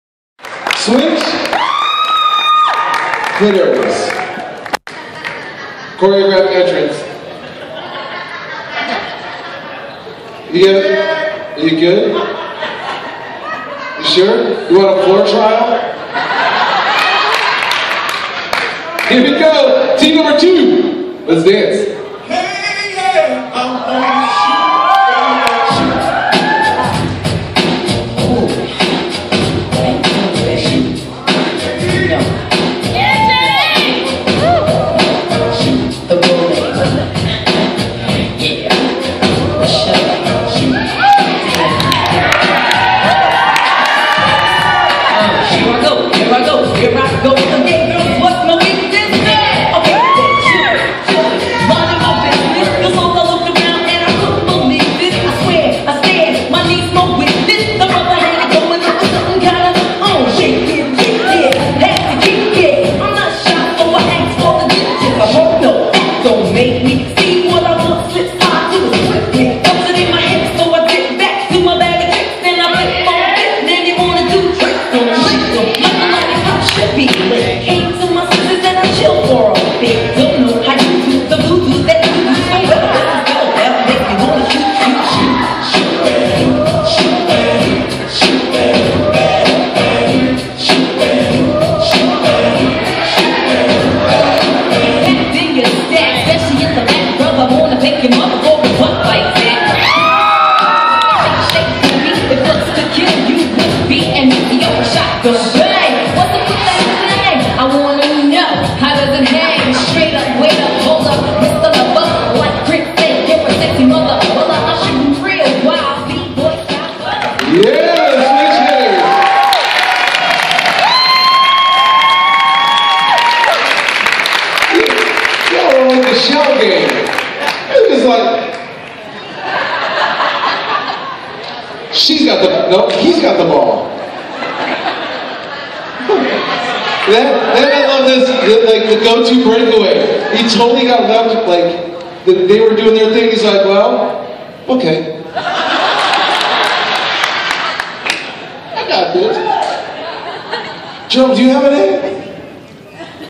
Threesome Contest 5 (JD, Sarah, Peter) :: Michigan Classic 2017
"Threesome with a Twist" contest spotlight with:
JD, Sarah, and Peter.

Michigan Classic on June 1-4, 2017.